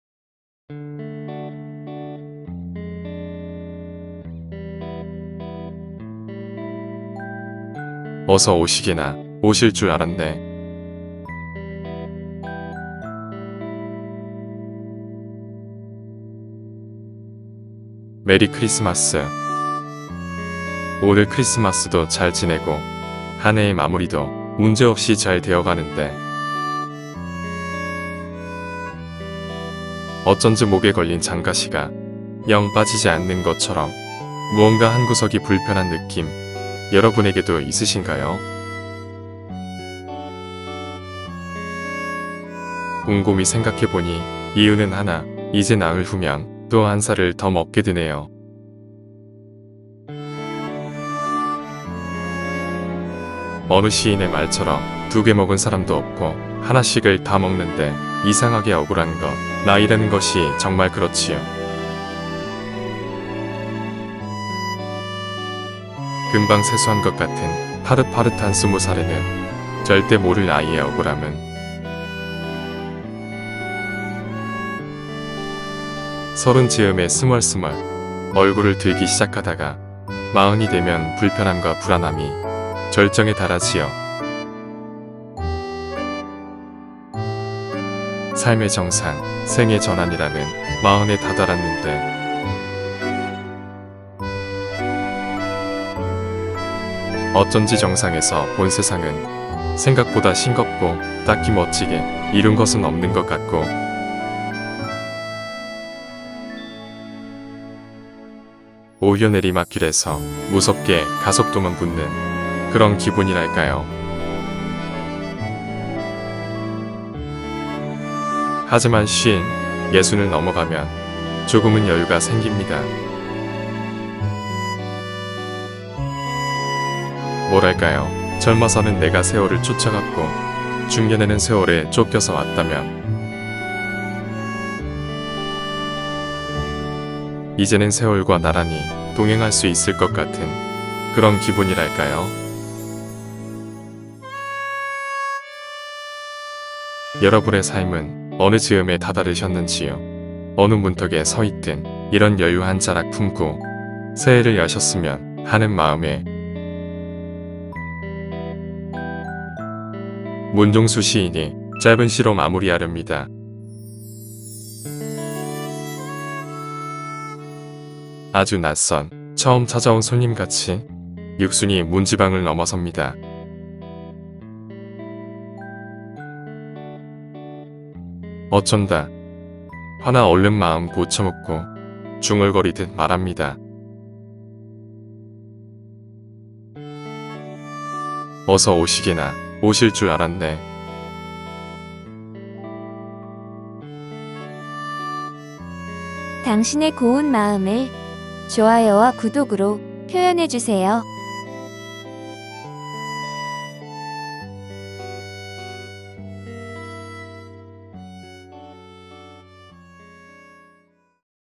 크리스마스와 새해 인사...여러분은 삶은 어느 즈음에 다다르셨는지요? 젊어서는 내가 세월을 쫓아갔고 중년에는 세월에 쫓겨서 왔지요?
메리 크리스마스!!! 젊어서는 내가 세월을 쫓아갔고 중년에는 세월에 쫓겨서 왔다면...이제는 세월과 나란히 동행할 수 있을 것 같은...그런 기분이랄까요?

여러분은 삶은 어느 즈음에 다다르셨는지요? 어느 문턱에 서 있든...여유 한 자락 품고...크리스마스를 보내고 새해를 여셨으면...